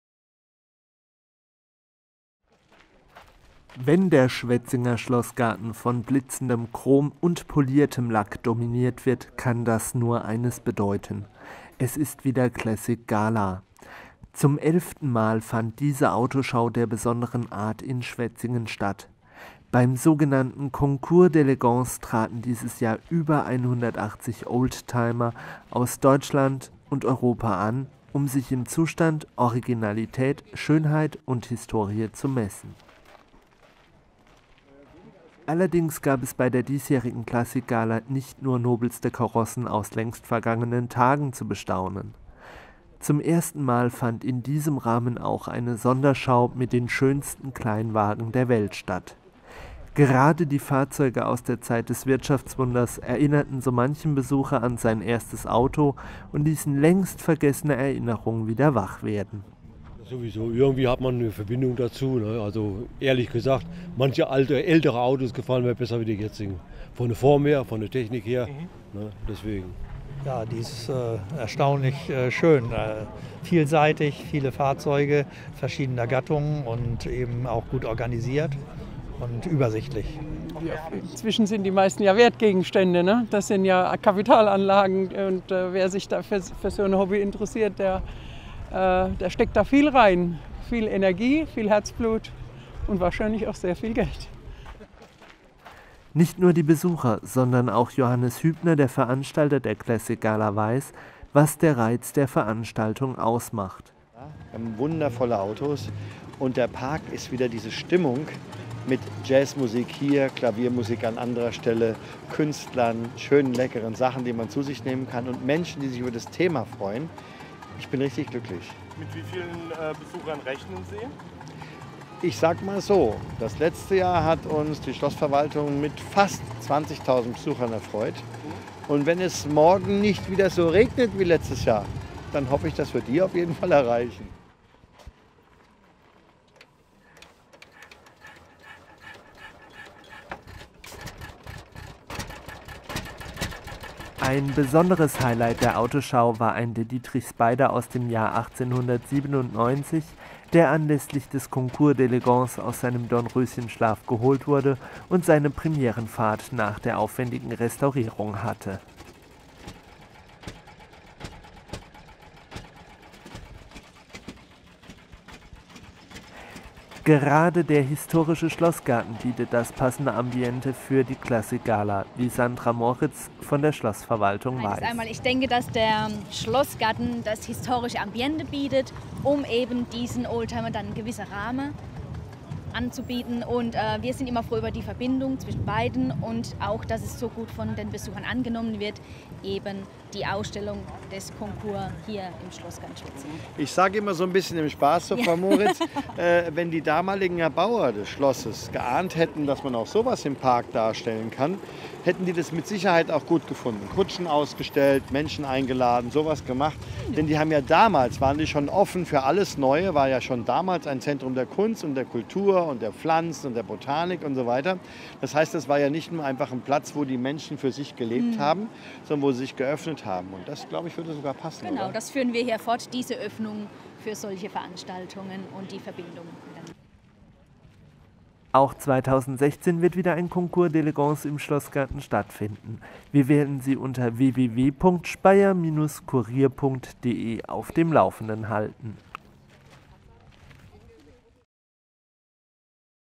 Concours d'Elegance Schwetzingen 2015
Concours d Elegance 2015 im Schloßgarten Schwetzingen.
Ein Film von Nico Stegmüller und Michael Waldschmidt im Auftrag des SPEYER-KURIER.